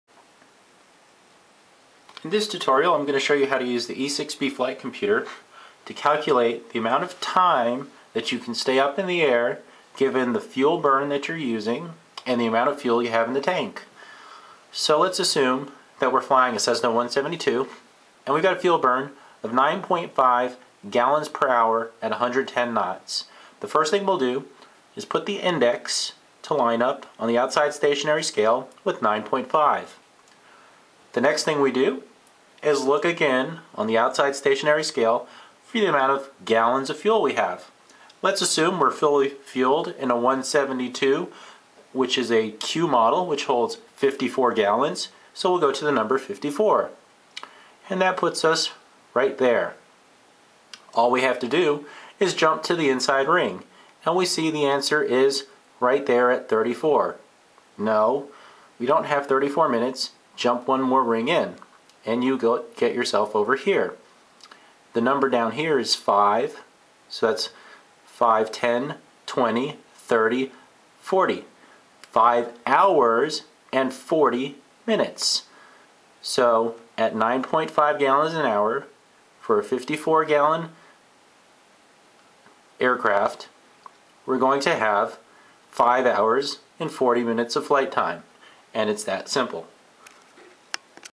E6B Flight Computer: Remaining Flight Time
In this tutorial, I will demonstrate how to calculate the amount of time the aircraft can stay in the air given the fuel remaining and fuel burn rate using the Jeppesen E6B mechanical flight computer. If you find any mistakes, please point them out. Good luck on your journey to becoming a pilot!